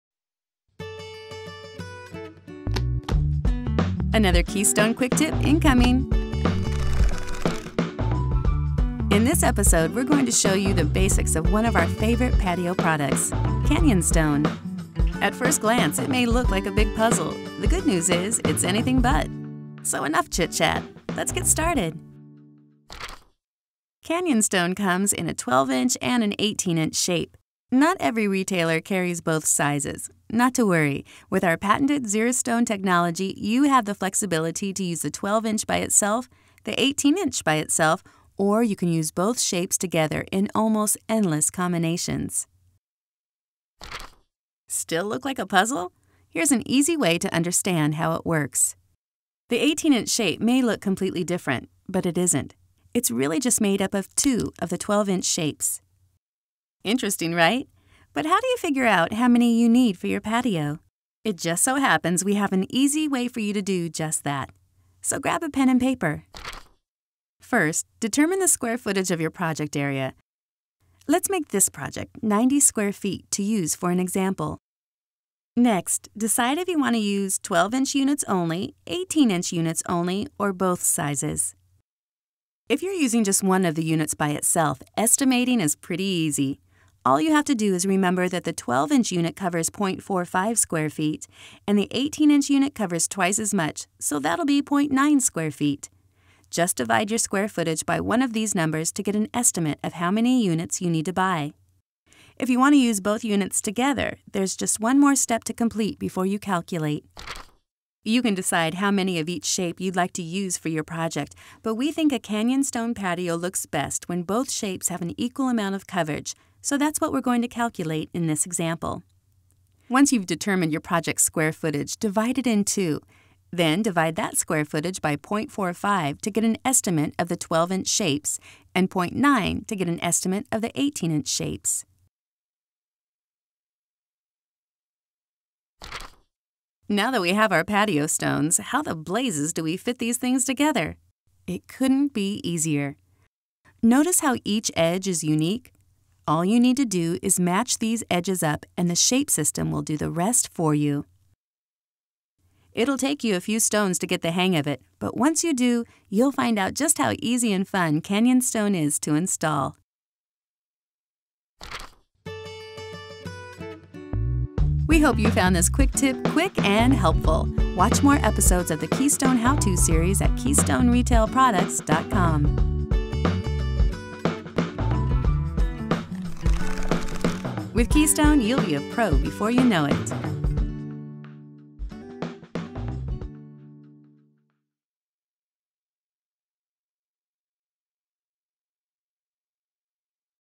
KEYSTONE CANYON STONE: Keystone Retaining Walls QUICK TIP -- Canyon Stone Basics
At first glance, Keystone's Canyon Stone patio product may look like a big puzzle. The good news is, it's anything but. In this Quick Tip episode, we'll show you just how easy it is to install and estimate the correct number Canyon Stone units needed, using one or both sizes of the patio stone. You'll see how Canyon Stone's unique shape system makes this patio stone easy and fun to install.